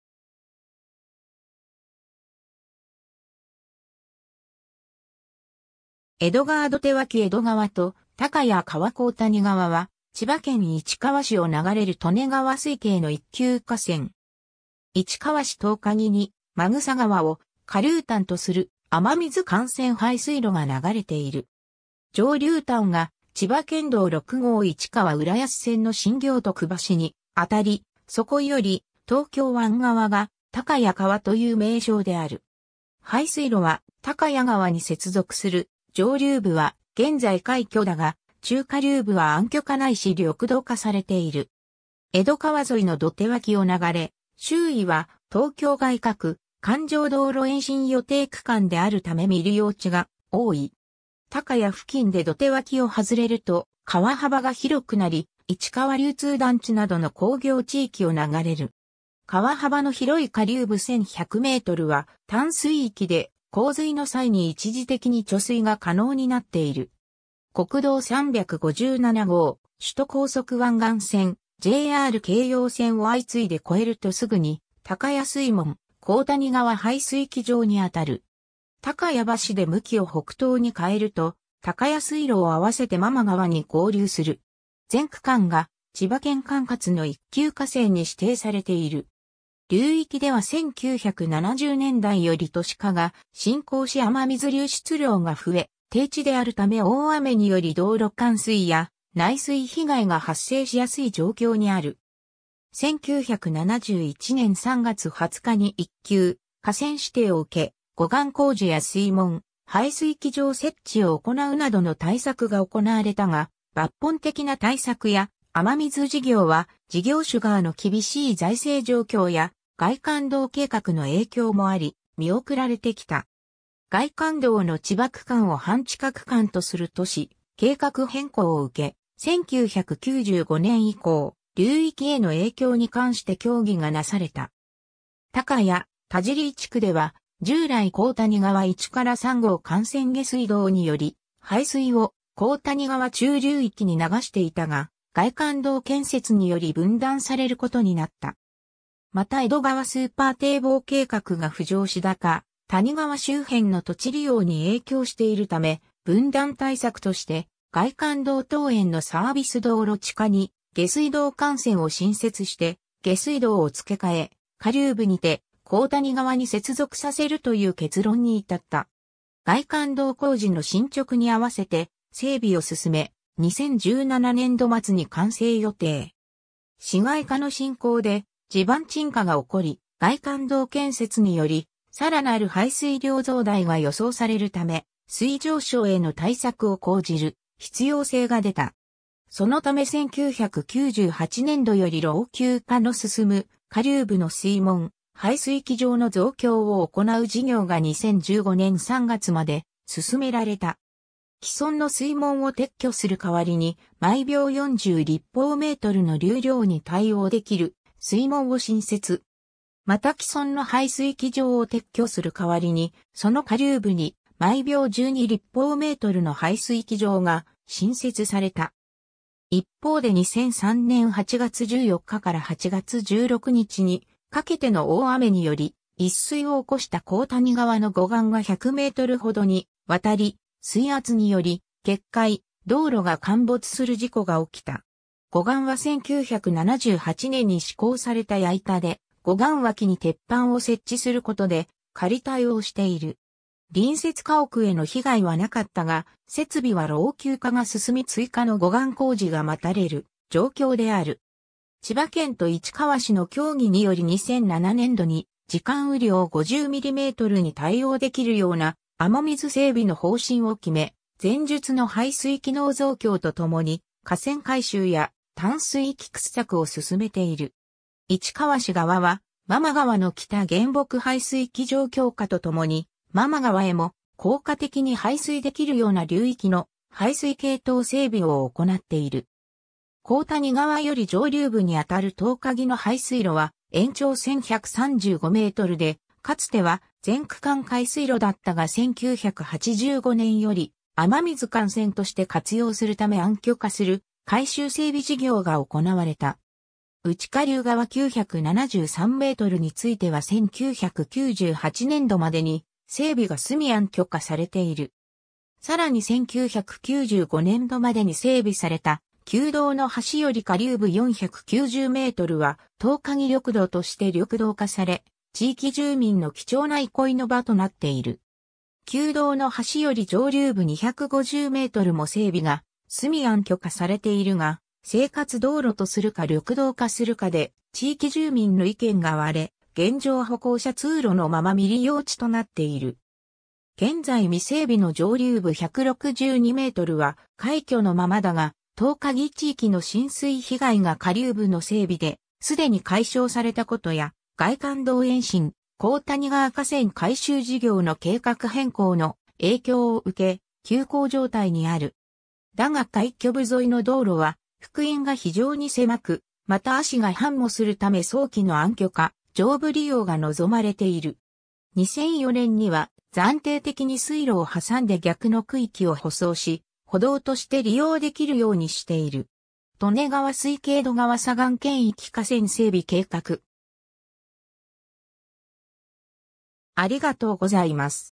高谷川 (利根川水系)
江戸川土手脇 江戸川（左）と高谷川（右） 高谷川（こうやがわ）は、千葉県市川市を流れる利根川水系の一級河川。
 市川市稲荷木に、秣川を下流端とする雨水幹線排水路が流れている。
上流端が千葉県道6号市川浦安線の新行徳橋にあたり、そこより東京湾側が「高谷川」という名称である。
排水路は高谷川に接続する上流部は現在開渠だが、中下流部は暗渠化ないし緑道化されている。
 江戸川沿いの土手脇を流れ、周囲は東京外かく環状道路（東京外環自動車道）延伸予定区間であるため未利用地が多い。
高谷付近で土手脇を外れると川幅が広くなり、市川流通団地などの工業地域を流れる。
川幅の広い下流部1,100mは湛水域で、洪水の際に一時的に貯水が可能になっている。
国道357号・首都高速湾岸線・JR京葉線を相次いで越えるとすぐに高谷水門・高谷川排水機場に当たる。
高谷橋で向きを北東に変えると、高谷水路を合わせて真間川に合流する。
全区間が千葉県管轄の一級河川に指定されている。
 流域では1970年代より都市化が進行し雨水流出量が増え、低地であるため大雨により道路冠水や内水被害が発生しやすい状況にある。
1971年（昭和46年）3月20日に一級河川指定を受け、護岸工事や水門・排水機場設置を行うなどの対策が行われたが、抜本的な対策や雨水事業は事業主（千葉県）側の厳しい財政状況や外環道計画の影響もあり見送られてきた。
 外環道の千葉区間を半地下区間とする都市計画変更を受け、1995年（平成7年）以降流域への影響に関して協議がなされた。
高谷・田尻地区では従来高谷川1 - 3号幹線下水道により排水を高谷川中流域に流していたが、外環道建設により分断されることになった。
また江戸川スーパー堤防計画が浮上し高谷川周辺の土地利用に影響しているため、分断対策として外環道東縁のサービス道路地下に下水道幹線を新設（名称上は高谷川1号幹線下水道を外環道に沿って南伸）して下水道を付け替え、下流部にて高谷川に接続させるという結論に至った。
外環道工事の進捗に合わせて整備を進め、2017年（平成29年）度末に完成予定。
 市街化の進行で地盤沈下が起こり、外環道建設により更なる排水量増大が予想されるため、水位上昇への対策を講じる必要性が出た。
そのため1998年（平成10年）度より老朽化の進む下流部の水門・排水機場の増強を行う事業（総事業費8,000万円）が2015年（平成27年）3月まで進められた。
既存の水門を撤去する代わりに、毎秒40立方メートルの流量に対応できる水門を新設。
また既存の排水機場（毎秒7.4立方メートル）を撤去する代わりに、その下流部に毎秒12立方メートルの排水機場が新設された。
 一方で2003年（平成15年）8月14日から8月16日にかけての大雨により、溢水を起こした高谷川の護岸が100m程にわたり水圧により決壊、道路が陥没する事故が起きた。
護岸は1978年（昭和53年）に施工された矢板で、護岸脇に鉄板を設置することで仮対応している。
隣接家屋への被害はなかったが、設備は老朽化が進み追加の護岸工事が待たれる状況である。
千葉県と市川市の協議により2007年（平成19年）度に時間雨量50mmに対応できるような雨水整備の方針を決め、前述の排水機能増強とともに河川改修や湛水域掘削を進めている。
市川市側は真間川の北原木排水機場強化とともに、真間川へも効果的に排水できるような流域の排水系統整備を行っている。
 高谷川より上流部にあたる稲荷木の排水路は延長1,135mで、かつては全区間開水路だったが1985年（昭和60年）より雨水幹線として活用するため暗渠化する改修整備事業が行われた。
うち下流側973mについては1998年（平成10年）度までに整備が済み暗渠化されている。
更に1995年（平成7年）度までに整備された旧道の橋より下流部490mは「稲荷木緑道」として緑道化され、地域住民の貴重な憩いの場となっている。
旧道の橋より上流部250mも整備が済み暗渠化されているが、生活道路とするか緑道化するかで地域住民の意見が割れ、現状歩行者通路のまま未利用地となっている。
 現在未整備の上流部162mは開渠のままだが、稲荷木地域の浸水被害が下流部の整備で既に解消されたことや、外環道延伸・高谷川河川改修事業の計画変更の影響を受け休工状態にある。
だが開渠部沿いの道路は幅員が非常に狭く、またアシが繁茂するため早期の暗渠化・上部利用が望まれている。
2004年（平成16年）には暫定的に水路...